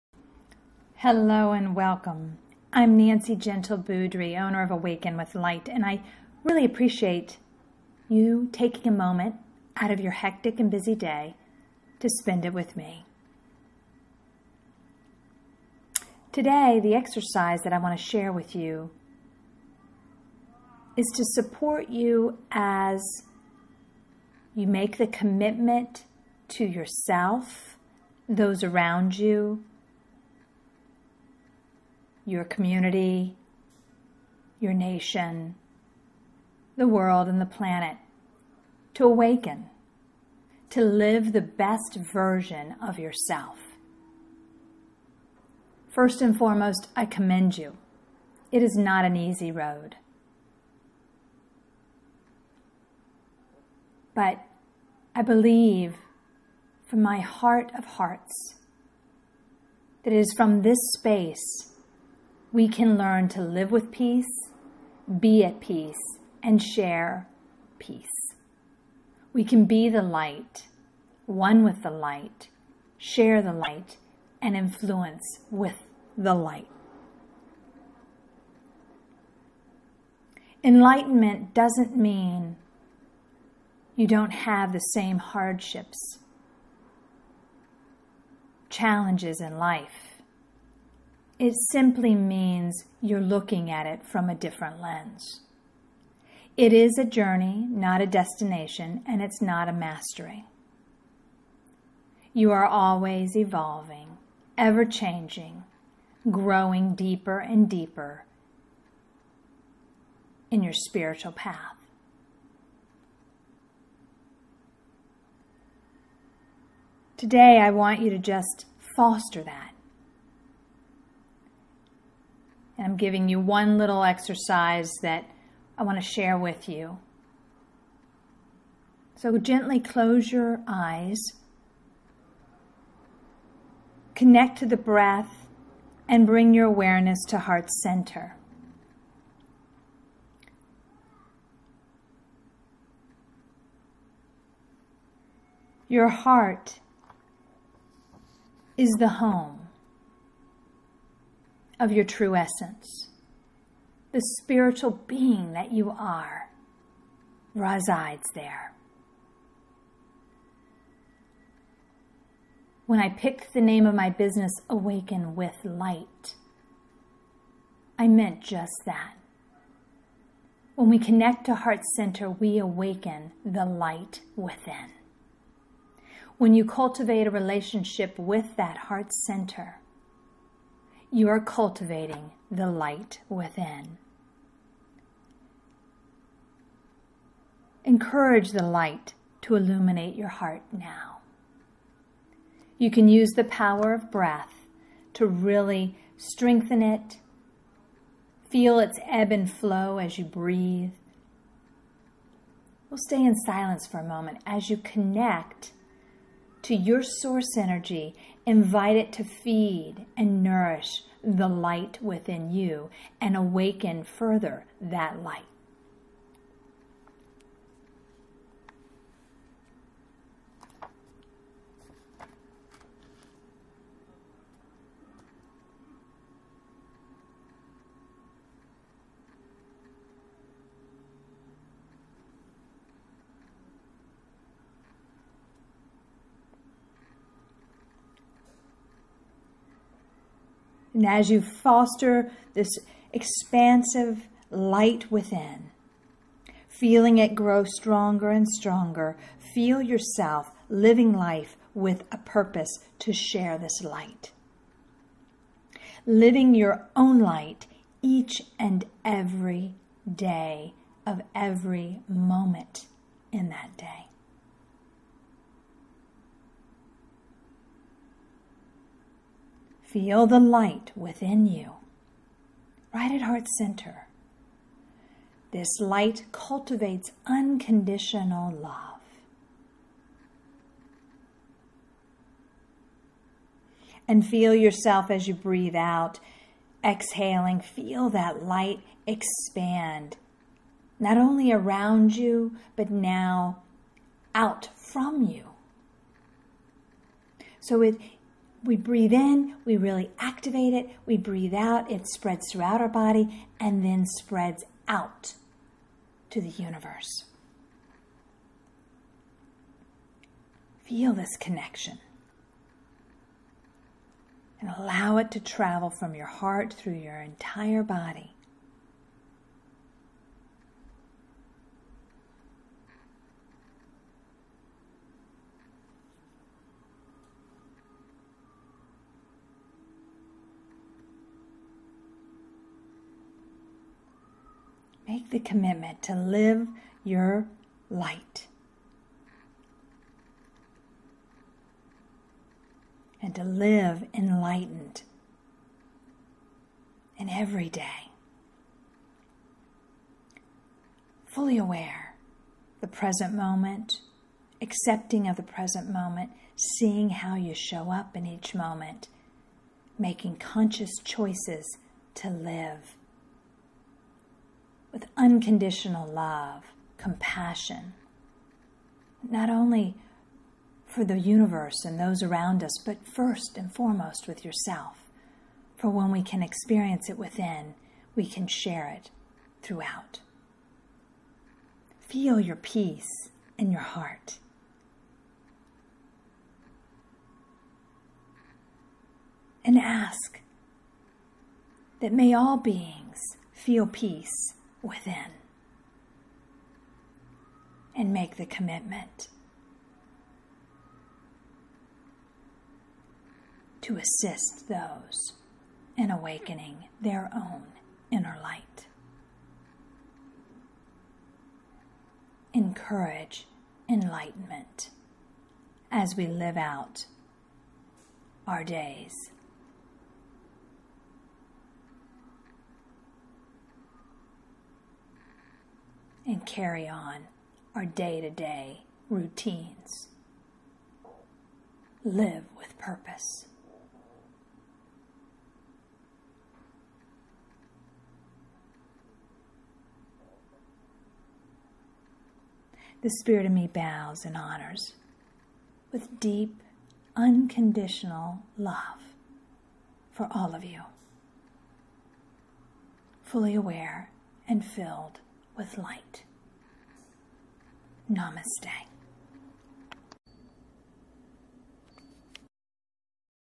Awaken Your Inner Light
A guided meditation to Awaken Your Inner Light, to help you connect to breath, tap into your source energy and connect to heart center to feel the light within you. Live your light and enjoy your life purpose. Meditations, inspiration, enlightening words from Nancy Gentle Boudrie of Awaken With Light, Inc.

I hope you enjoy this guided light awakening meditation video!